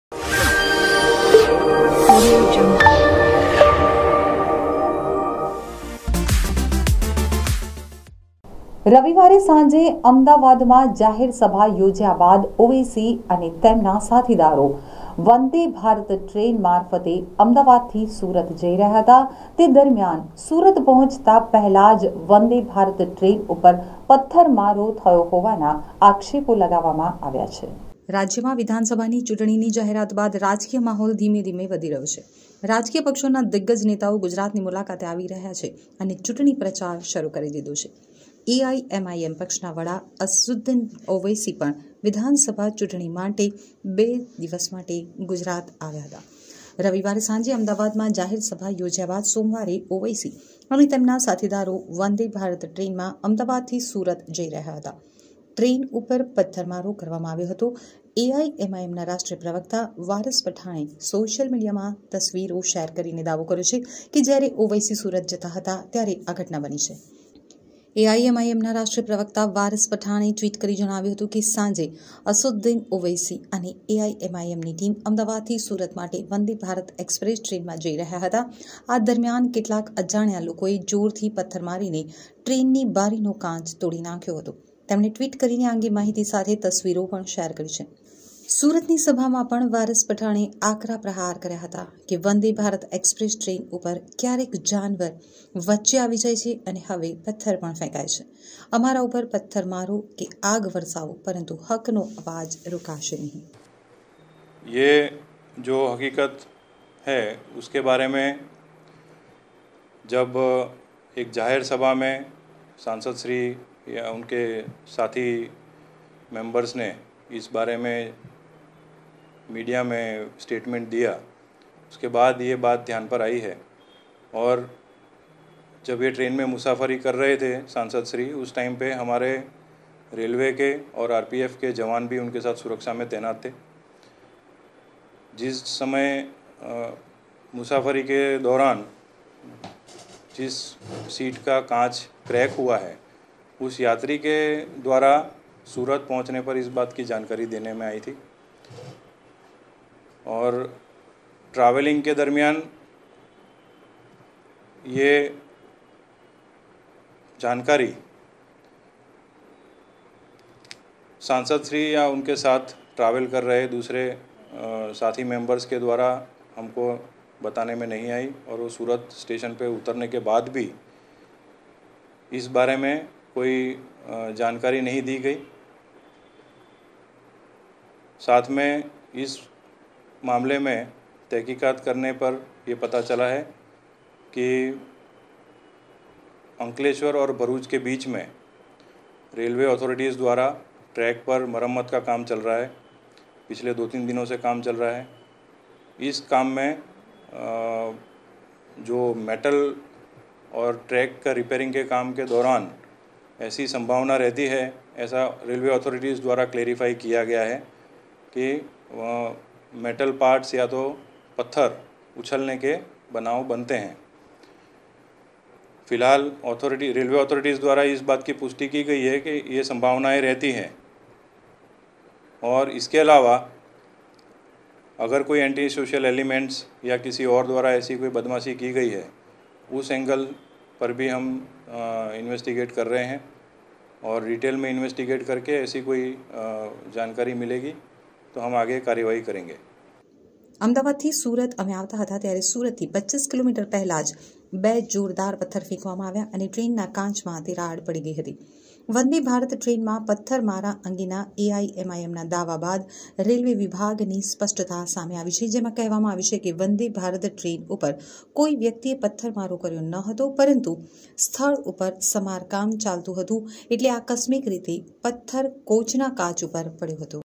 AIMIMના નેતા વારીસ પઠાણના આક્ષેપનો મામલો
વંદે ભારત ટ્રેનના કોચ પર પથ્થર મારવાની ઘટના